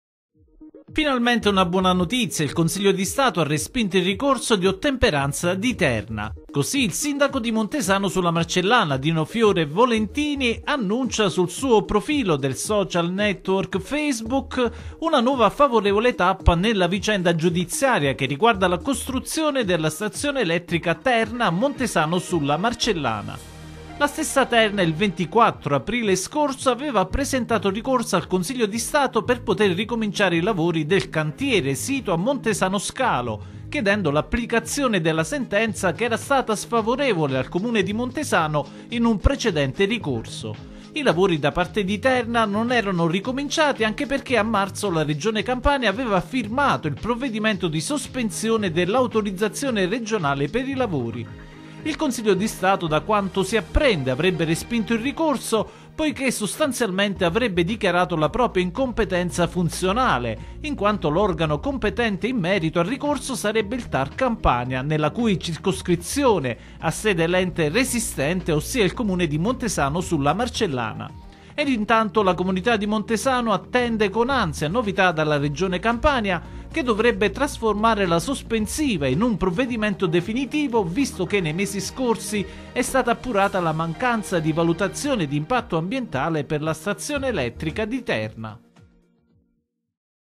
Montesano S/M (SA): il Consiglio di Stato respinge il ricorso di Terna
29/10/2014